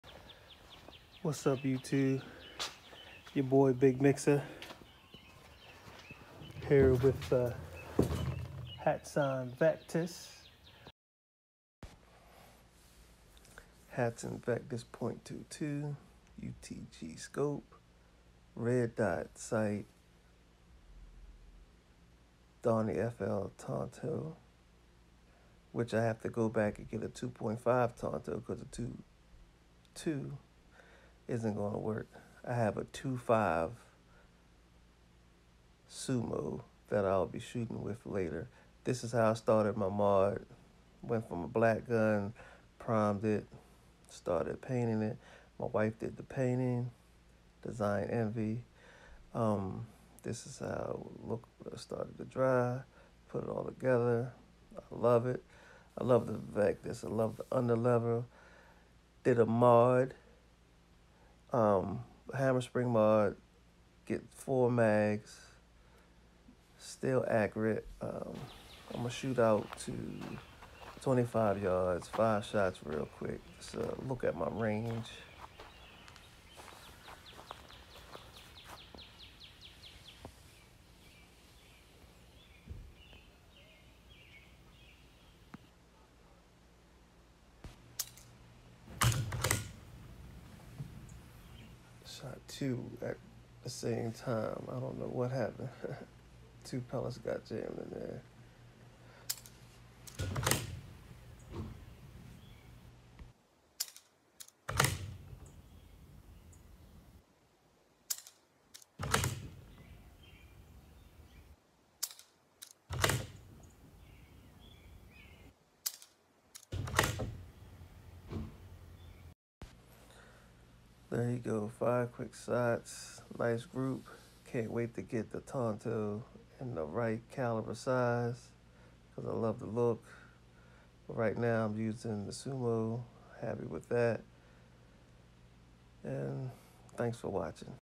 Custom painted stock Hatsan vectis .22
Quick look longer video coming soon Custom painted rifle stock Hatsanvectis .22 with Donty FL modulator